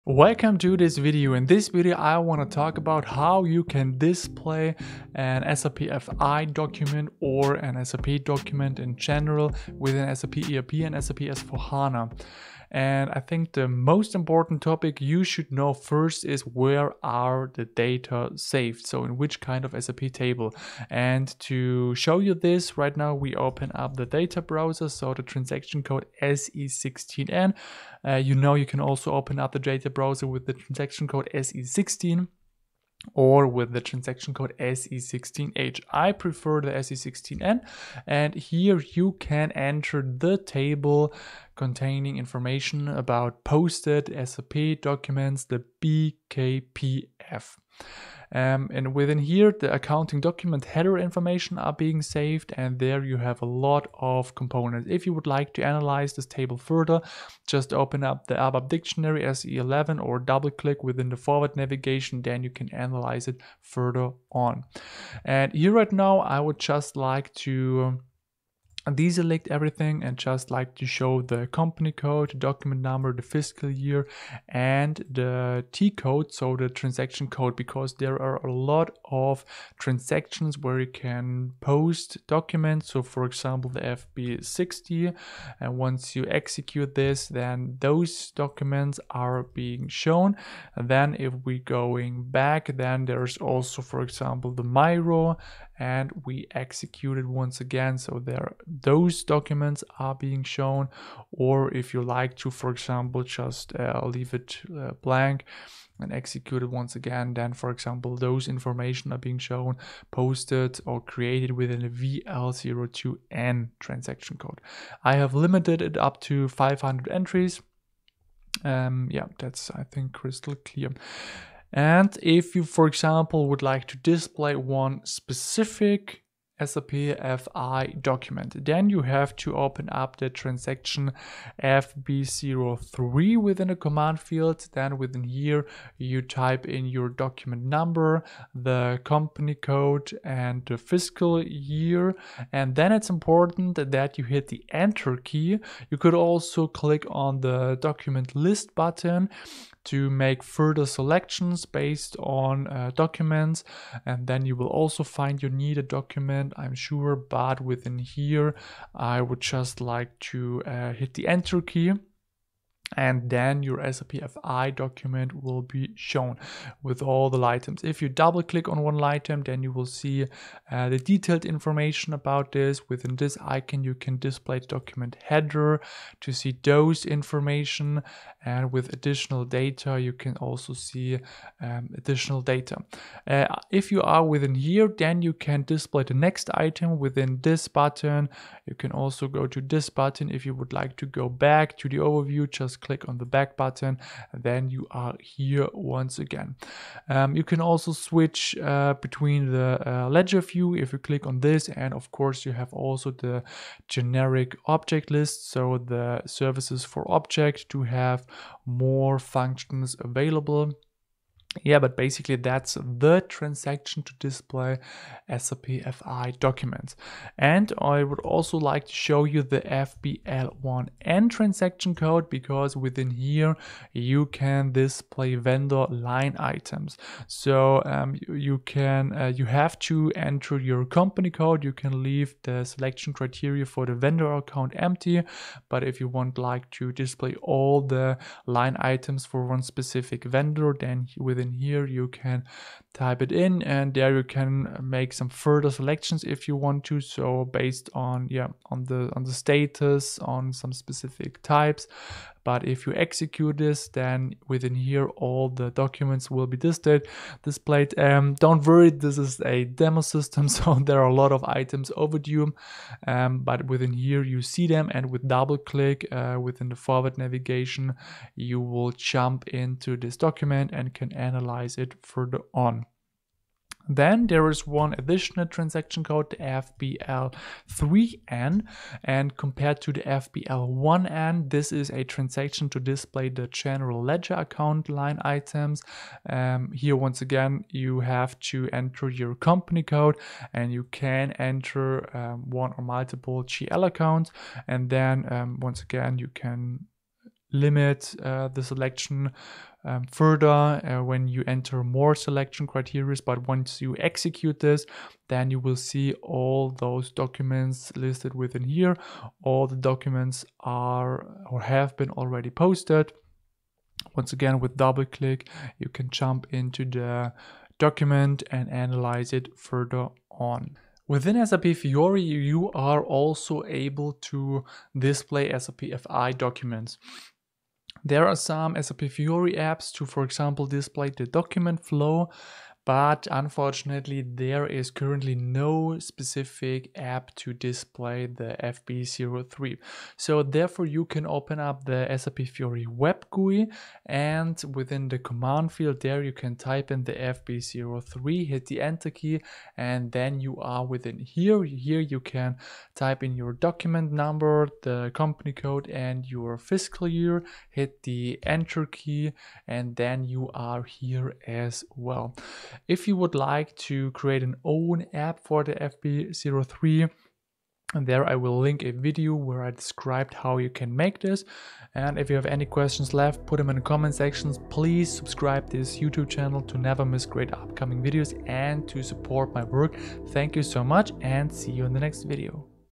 How to Display SAP Documents in SAP FI 🌟
In this tutorial, discover the step-by-step process to efficiently display SAP documents in SAP FI (Financial Accounting). Learn how to navigate and access essential documents, enabling streamlined financial management within the SAP system. You will also learn how to access documents in SAP Fiori.

▬ Content ▬▬▬▬▬▬▬▬▬▬▬▬
00:00 - Intro
00:12 - Table BKPF
02:01 - Display SAP FI Document - FB03
03:37 - Display Vendor Line Item - FBL1N
04:33 - Display G/L Account Line Item - FBL3N
05:20 - Display Documents in SAP Fiori
06:11 - Outro